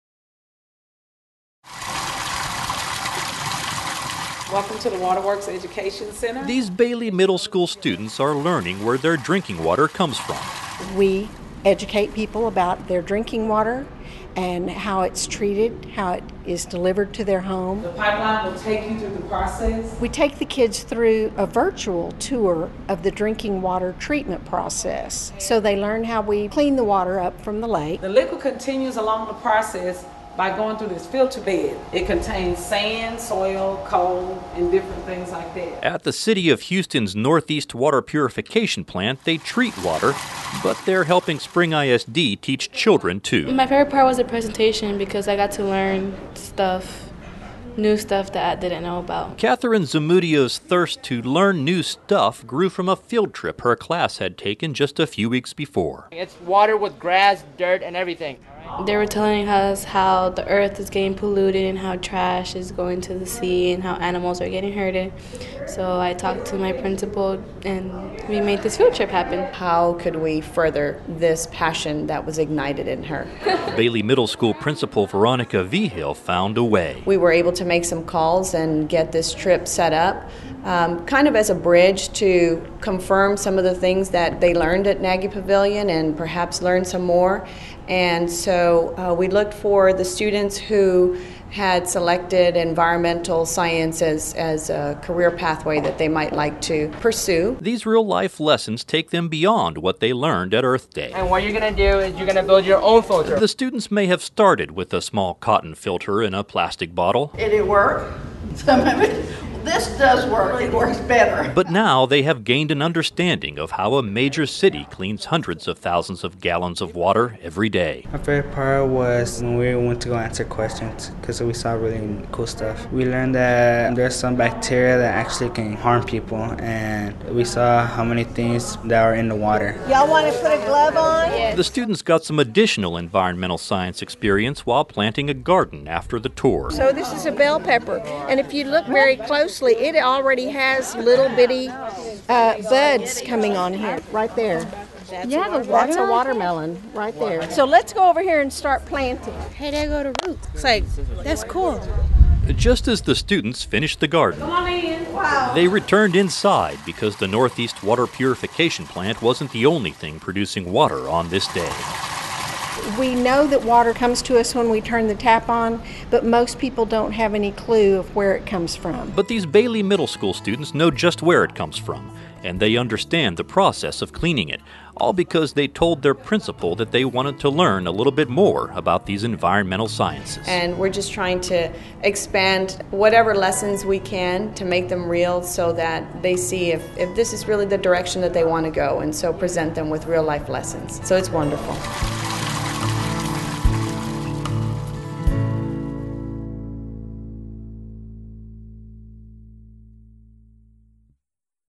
Spring ISD Students Learn About Environmental Sciences at The WaterWorks Education Center
Bailey Middle School students studying environmental sciences visited The WaterWorks Education Center to learn about our water supply.